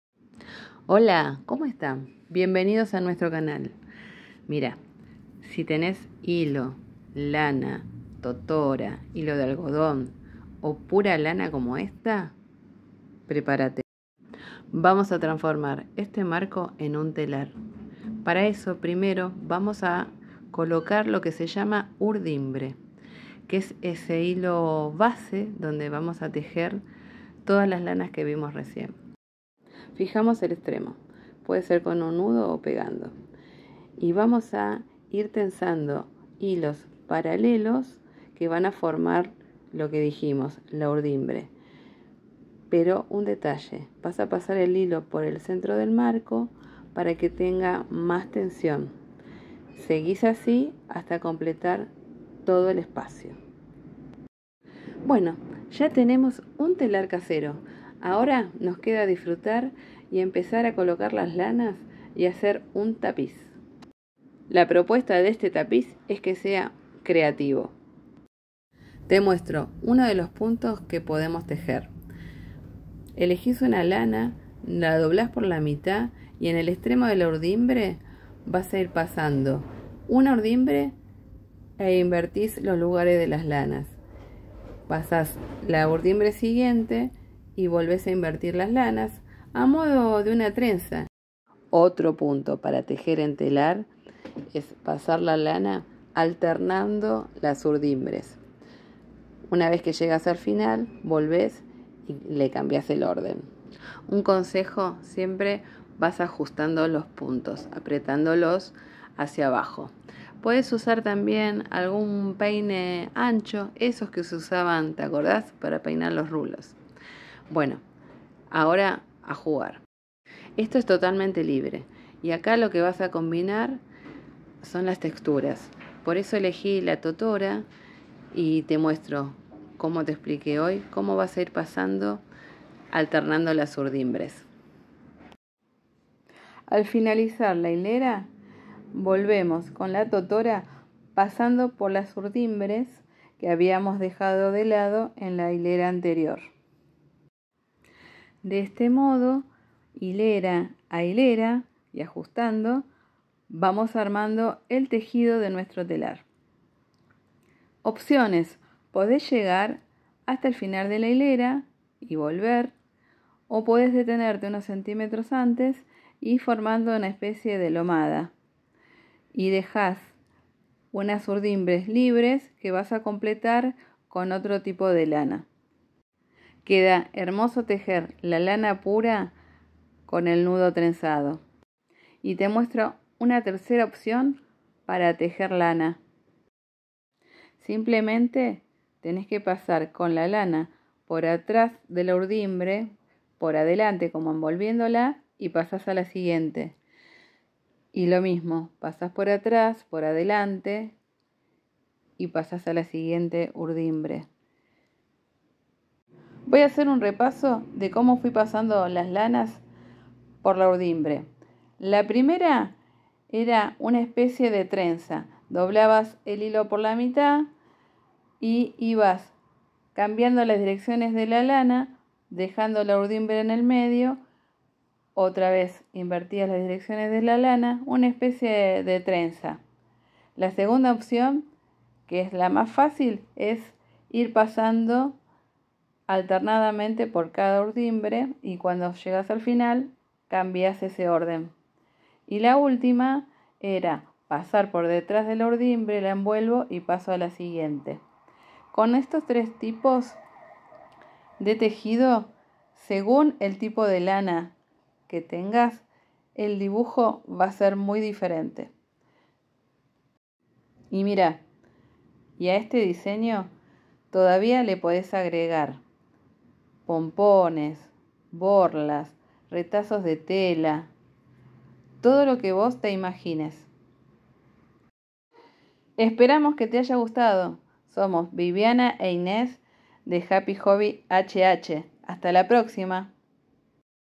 DIY: Haz tu propio telar de manera creativa!!handmade
Hola! Si tenes sobrantes de lanas, y un marco de madera no te pierdas este tapiz deco.